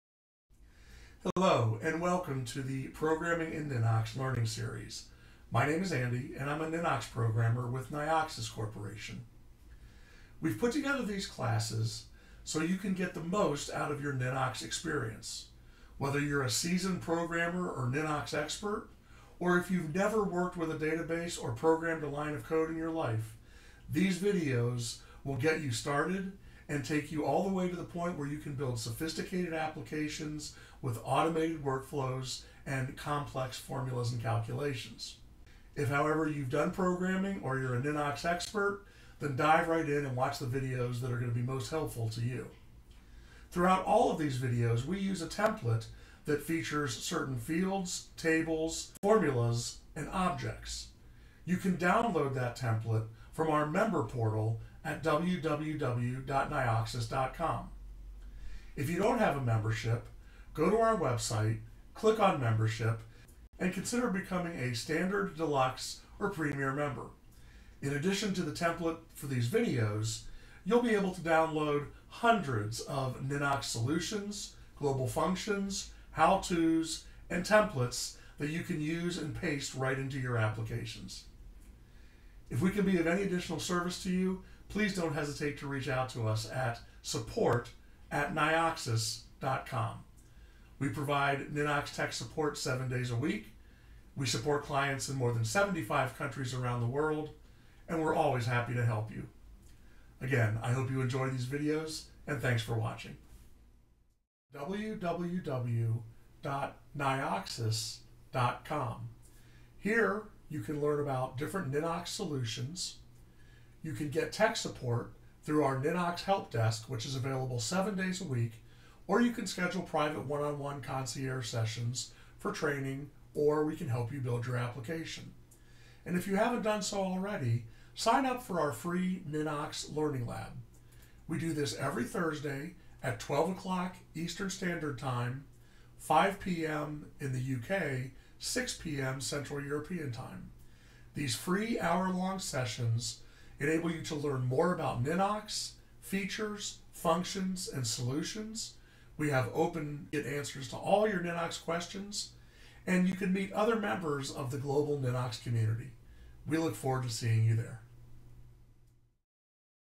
Welcome to Programming in Ninox
The Programming in Ninox learning series can help you get the most out of your Ninox Database solution.  Whether you have worked with Ninox already or are getting ready to write your first line of code or design your first Ninox application, this learning series will help you understand how to get the most out of your Ninox application.